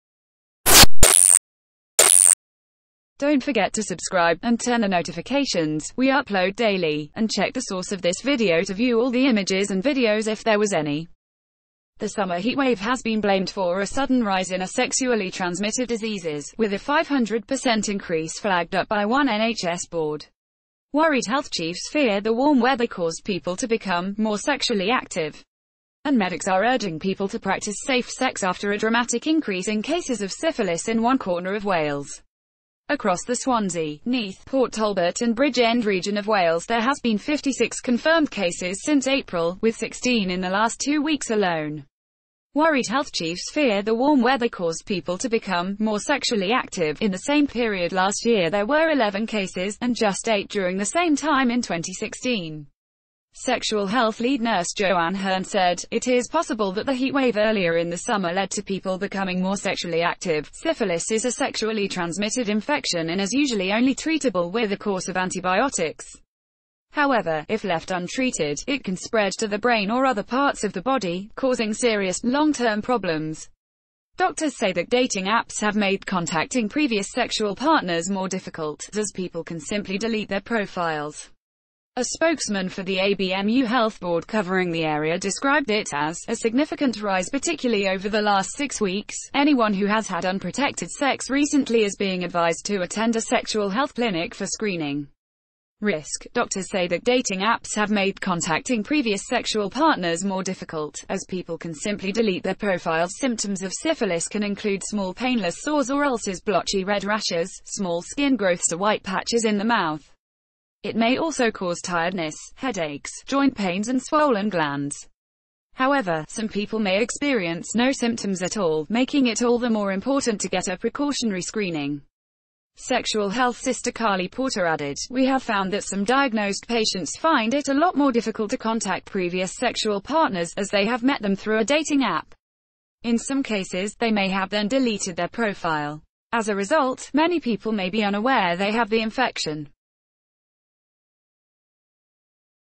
BREAKING! Summer heatwave blamed for 500 per cent spike in syphilis infections
Source DONT' FORGET TO SUBSCIBE TO OUR CHANNEL The summer heatwave has been blamed for a sudden rise in a sexually-transmitted diseases - with a 500 per cent increase flagged up by one NHS board.

Worried health chiefs fear the warm weather caused people to become 'more sexually active'.

And medics are urging people to practice safe sex after a dramatic increase in cases of syphilis in one corner of Wales.

Across the Swansea, Neath, Port Tal ...